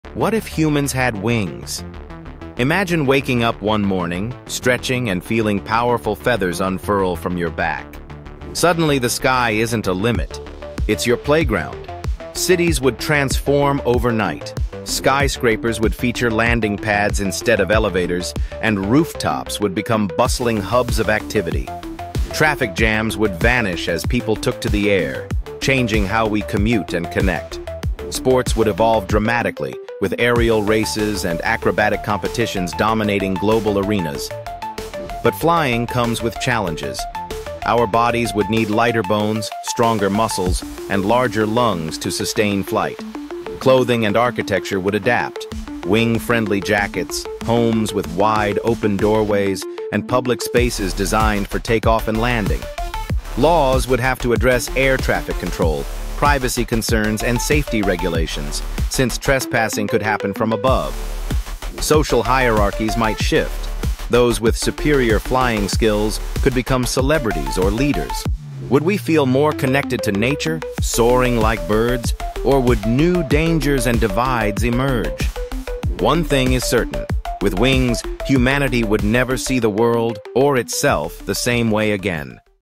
What If Humans Grew Wings Overnight?
What if you woke up tomorrow… and found wings on your back? 🕊️
A world where humans could fly would change everything — from how cities are built to how we travel, work, and live. Skies filled with people, rooftop highways, and airborne sports — it sounds beautiful, but would it also create chaos?

In this episode of What If…, we explore the science, sociology, and imagination behind a winged humanity. Could evolution really make it possible — or would gravity win in the end?